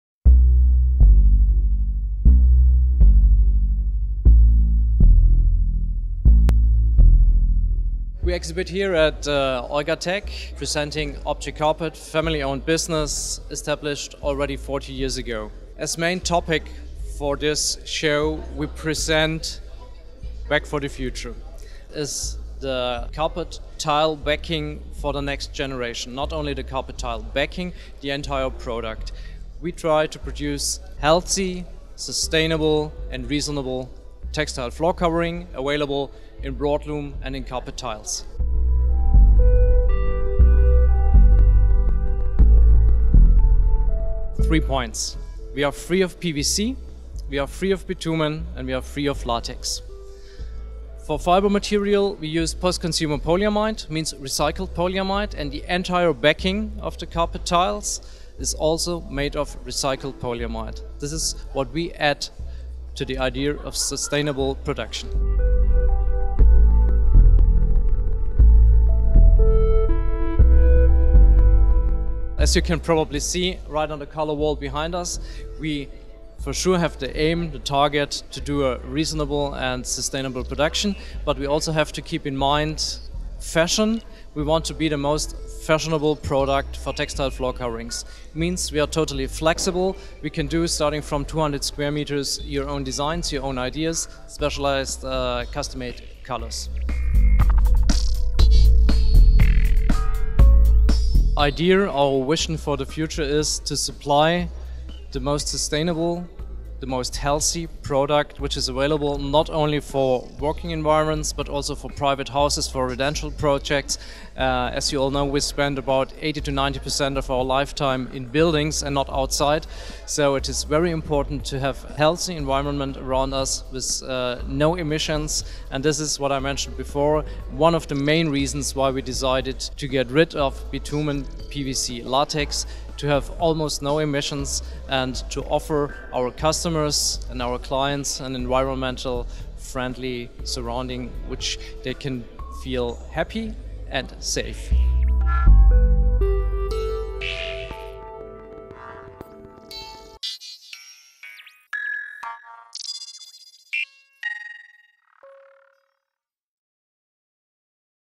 Orgatec 2018 | OBJECT CARPET - Frank Zimmermann presents Welltex, the new tiles backing
Export Director Frank Zimmerman presents WELLTEX, the new tiles backing by Objet Carpet. Interview by Archiproducts during Orgatec 2018 in Cologne.